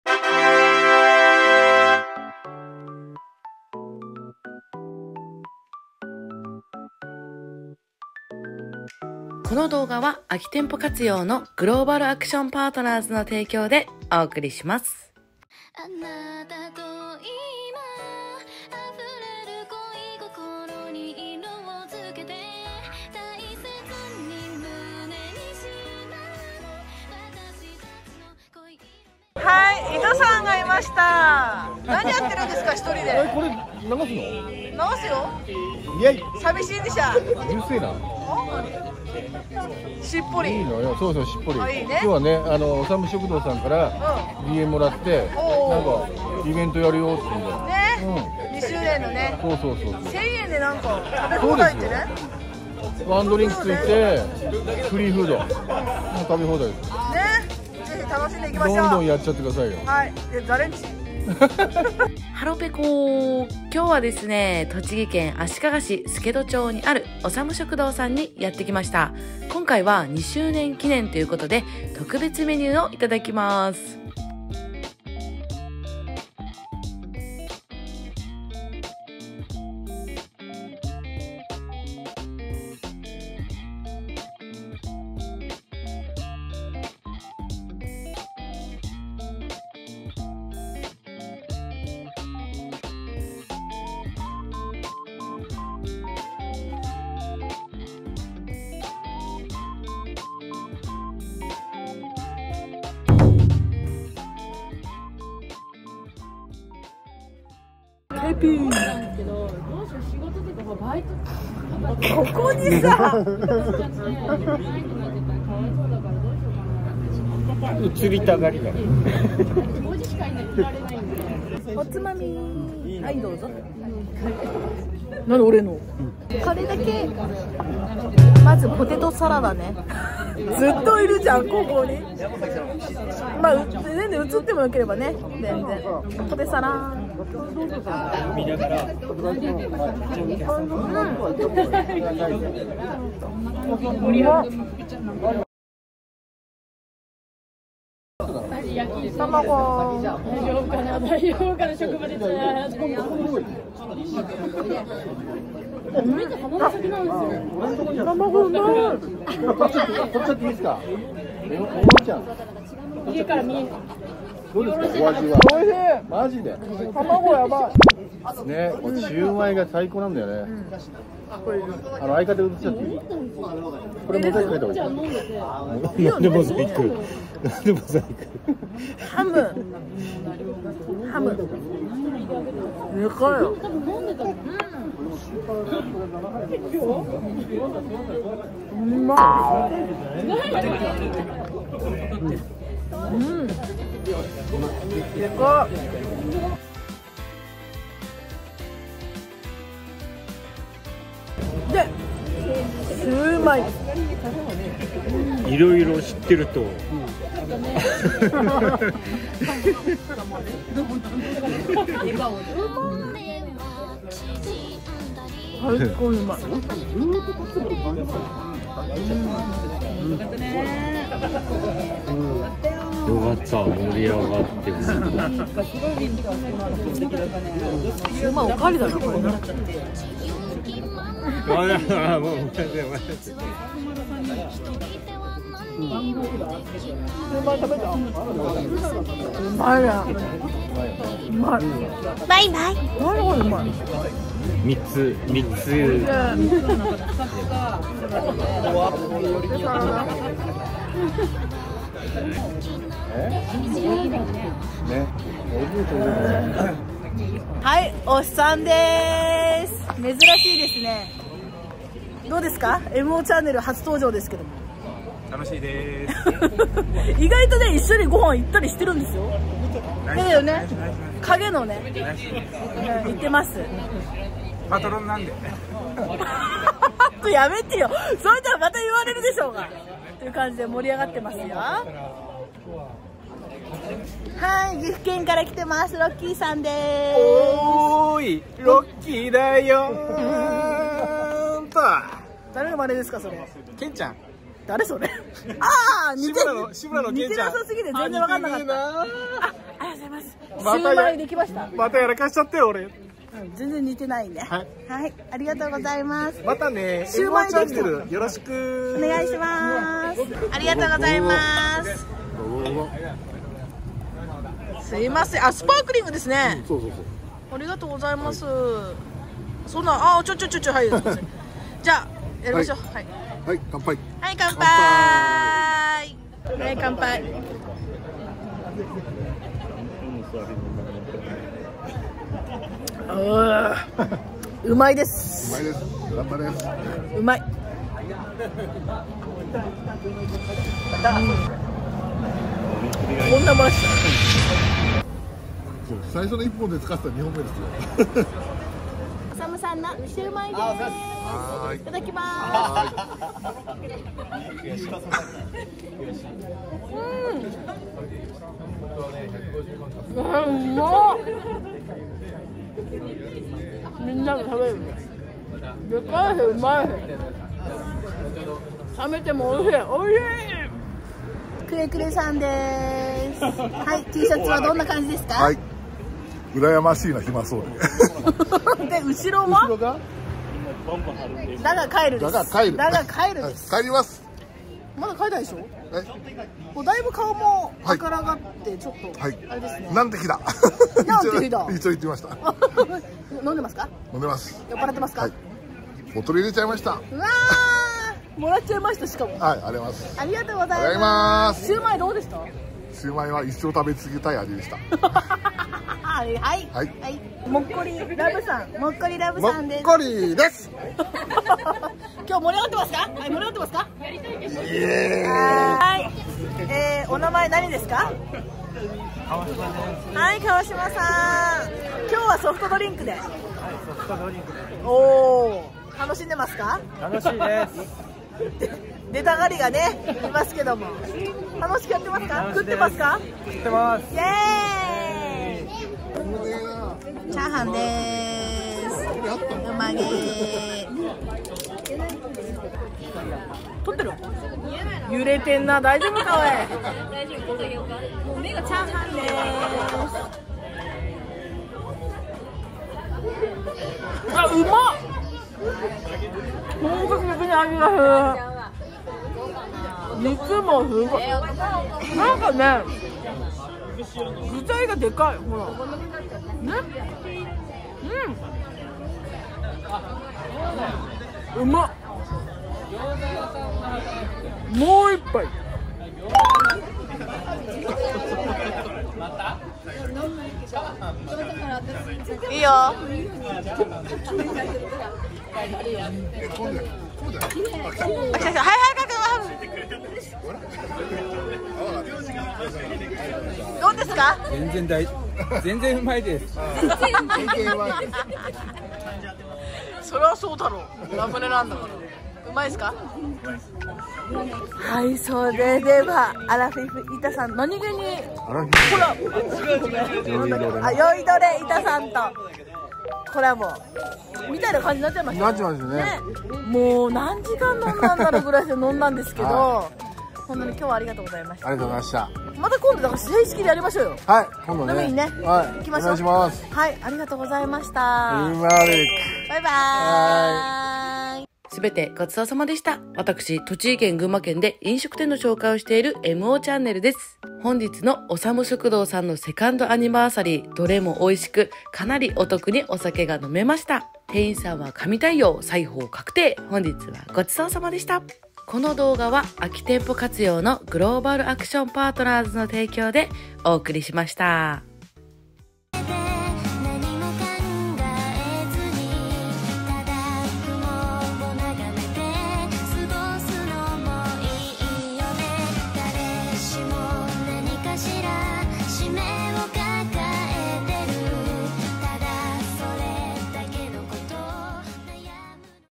【足利市】おさむ食堂💚２周年記念で腹ぺこ御一行と楽しくワイワイ呑んできました🍺1000円でワンドリンク付ブッフェ食べ放題はお得すぎました🤙☀️やはりシュウマイは旨すぎる🥹🥹🙌
おさむ食堂様
住所/栃木県足利市助戸1-614-1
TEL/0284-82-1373
定休日/木曜日
営業時間/18:00 - 00:00
L.O. 23:00

このMOちゃんねるではリスナー様のことを
「腹ぺこちゃん」と呼んでいます💚
なので「腹ぺこちゃん」って呼んだら反応してくださいね💚